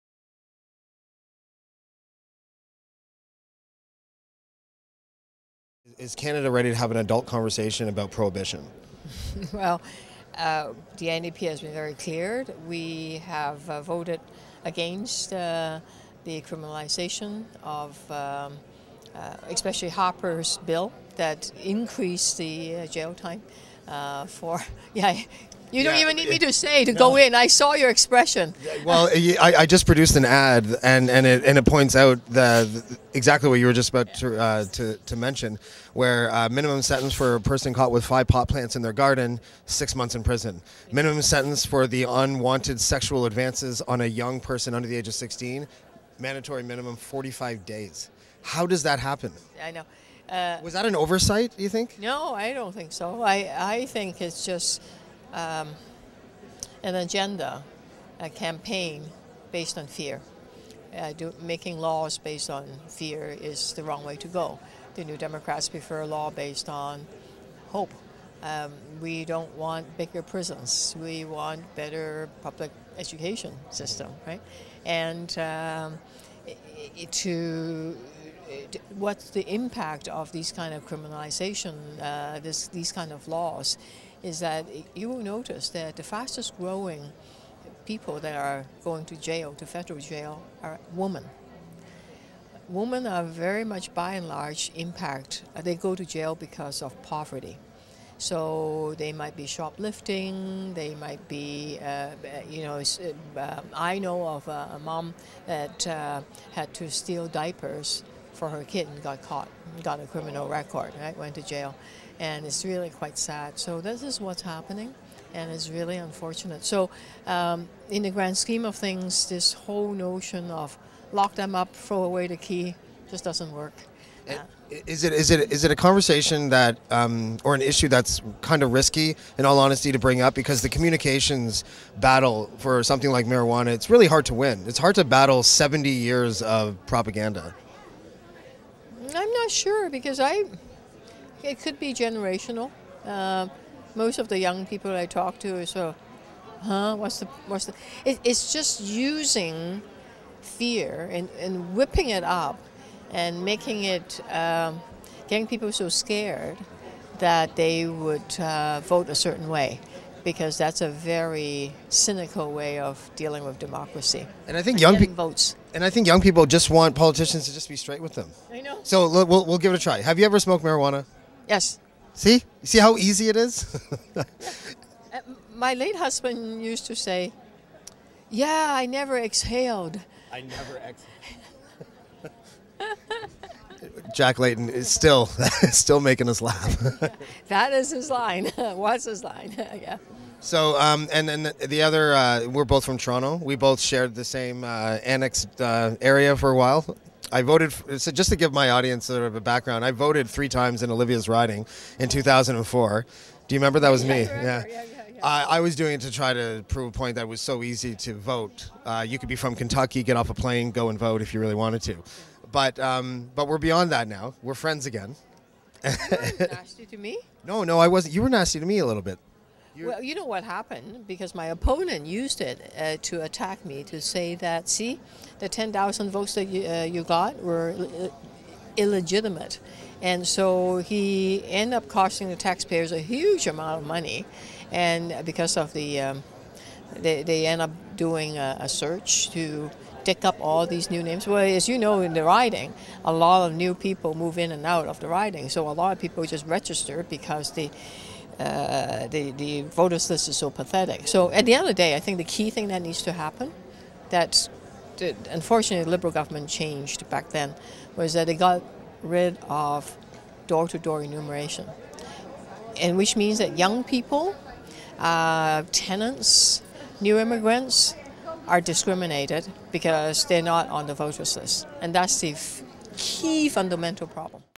Olivia Chow crashes the Liberal Convention
Red Dot Project finds NDP MP Olivia Chow roaming the Liberal Convention.